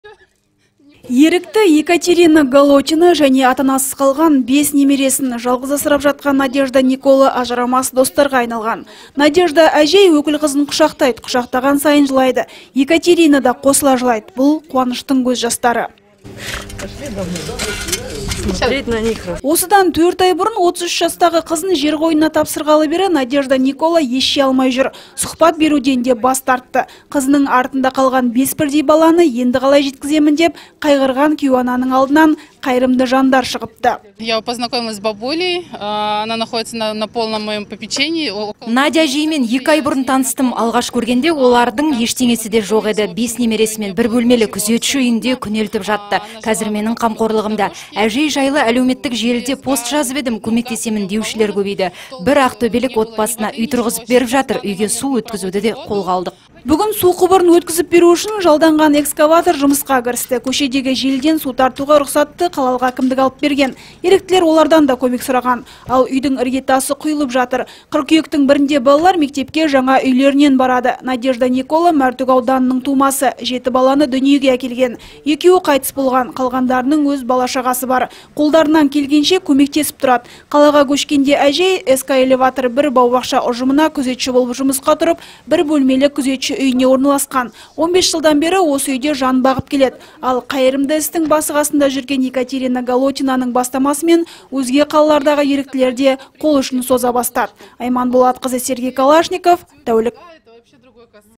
Еріктілер зейнеткерге үй салып жатыр
Еріктілер ата-анасыз қалған 5 балаға үй тұрғызып беріп жатыр. 64 жастағы зейнеткер әжесінің қамқорлығында қалған балалардың ең үлкені 15 жаста, кенжесі 5-те. Қазір олар 1 бөлмелі күзетші үйінде жан бағып отыр. Ерікті Екатерина Голотина және ата-анасыз қалған 5 немересін жалғыз асырап жатқан Надежда Никола ажырамас достарға айналған. Надежда Әжей өкіл қызын құшақтайды. Құшақтаған сайын жылайды. Екатерина да қосыла жылайды. Бұл қуаныштың көз жастары. Осыдан 4 ай бұрын 33 жастағы қызын жер қойнына тапсырғалы бері Надежда Никола ес жия алмай жүр. Сұхбат  беруден де бас тартты.  Қызының артында қалған 5 бірдей баланы енді қалай жеткіземін деп қайғырған кейуананың алдынан қайырымды жандар шығыпты. Бүгін су құбырын өткізіп беру үшін жалданған эксковатор жұмысқа кірісті. Көшедегі желіден су тартуға рұқсатты қалалық әкімдік алып берген. Еріктілер олардан да көмек сұраған. Ал үйдің іргетасы құйылып жатыр. Қыркүйектің бірінде балалар мектепке жаңа үйлерінен барады. Надежда Никола Мәртөк ауданының тумасы. Жеті баланы дүниеге әкелген. Екеуі қайтыс болған. Қалғандарының өз бала-шағасы бар. Қолдарынан келгенше көмектесіп тұрады. Қалаға көшкенде әжей  "СК Элеватор-1" бау-бақша ұжымына күзетші болып жұмысқа тұрып, 1 бөлмелі күзетші үйіне орналасқан. 15 жылдан бері осы үйде жан бағып келеді. Ал қайырымды істің басы-қасында жүрген Екатерина Голотинаның бастамасымен өзге қалалардағы еріктілер де қол ұшын соза бастады.